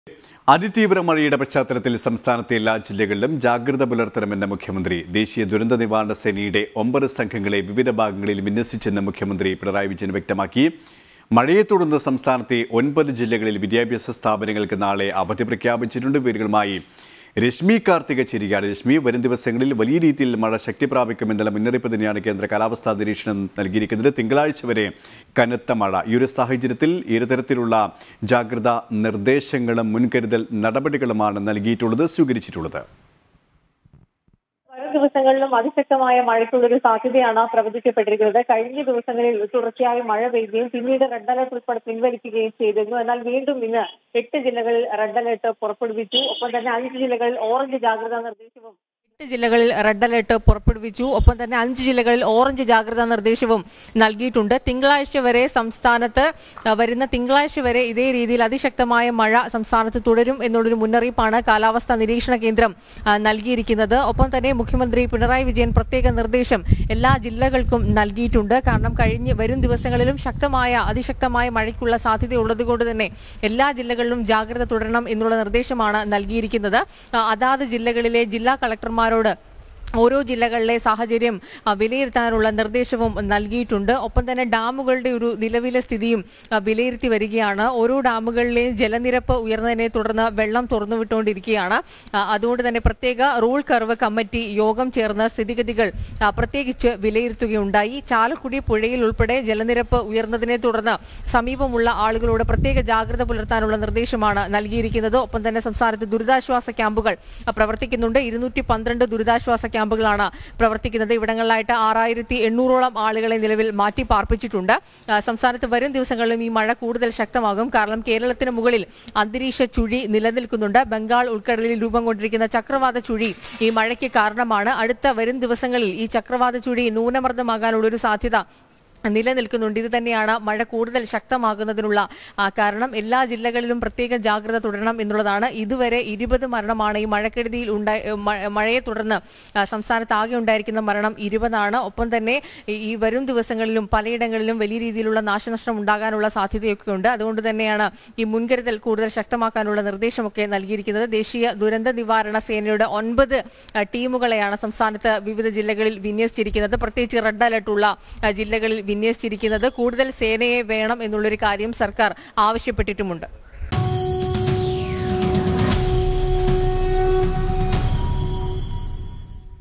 സംസ്ഥാനത്തെ എല്ലാ ജില്ലകളിലും ജാഗ്രത പുലര്‍ത്തണമെന്ന് മുഖ്യമന്ത്രി
സംസ്ഥാനത്തെ എല്ലാ ജില്ലകളിലും ജാഗ്രത പുലര്‍ത്തണമെന്ന് മുഖ്യമന്ത്രി. ദേശിയ ദുരന്ത നിവാരണ സേനയുടെ 9  സംഘങ്ങളെ വിവിധ ഭാഗങ്ങളില്‍ വിന്യസച്ചു